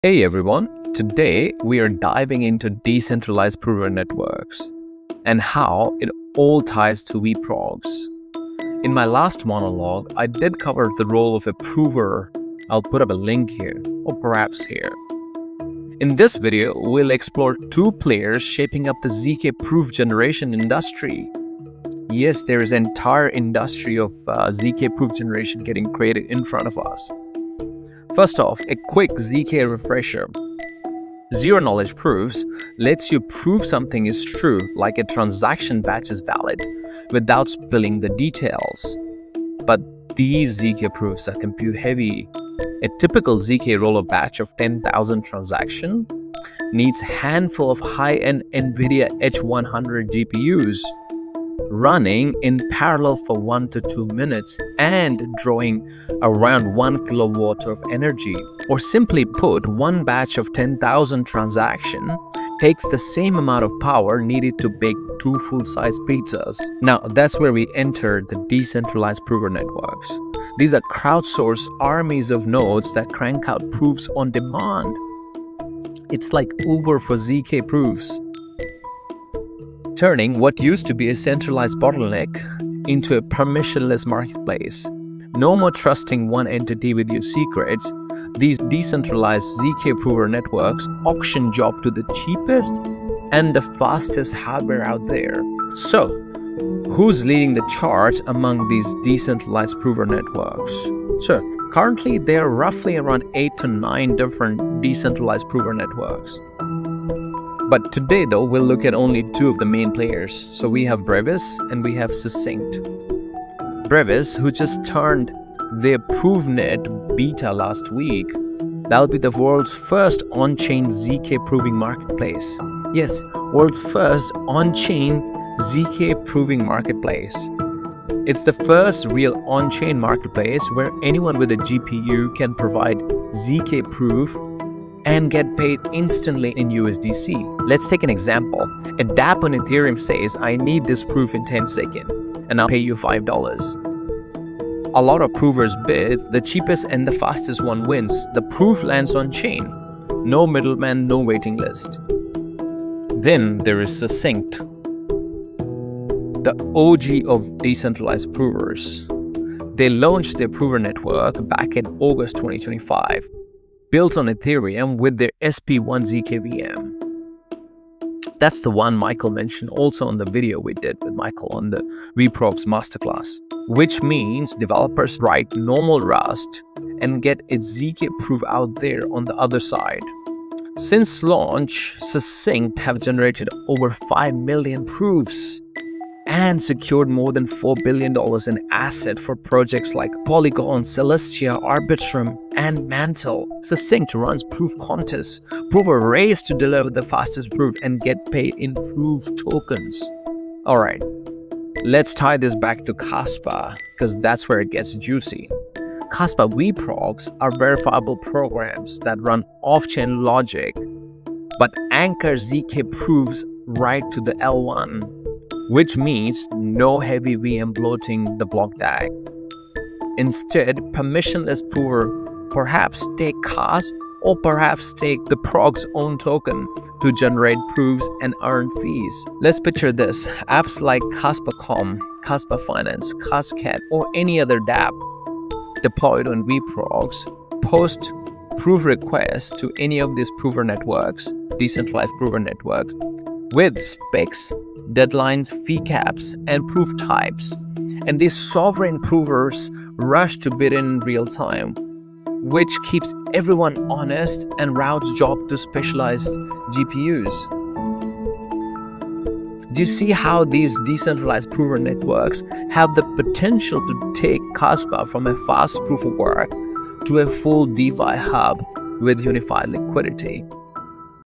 Zero Knowledge Prover Networks: KASPA from fast L1 to full DeFi Hub (4 Min)
In my previous monologue, I covered the critical role of provers in Zero Knowledge systems—today, we're diving into decentralized prover networks and how they perfectly tie into Kaspa's Verifiable Programs (vProgs), unlocking a new era for ZK-proof generation.

We start with a quick ZK refresher: Zero-knowledge proofs let you validate things like transaction batches without revealing details—but they're compute-intensive, requiring high-end GPUs and serious energy (think baking two full-size pizzas per 10,000-transaction batch!).

Enter decentralized prover networks: crowdsourced "armies" of nodes that generate proofs on demand—like Uber for ZK proofs—turning centralized bottlenecks into permissionless, auction-based marketplaces. No more trusting a single entity; jobs go to the fastest, cheapest hardware available.We zoom in on two leading players shaping this industry:

The real excitement lies in their synergy with Kaspa's Verifiable Programs (vProgs): by enabling permissionless provers to compete in real-time for off-chain proof requests from Kaspa dApps, these networks anchor tiny ZK proofs directly to the lightweight BlockDAG, keeping costs low, decentralization high, and paving the way for Kaspa to evolve from a high-speed PoW chain into a robust DeFi hub with unified liquidity.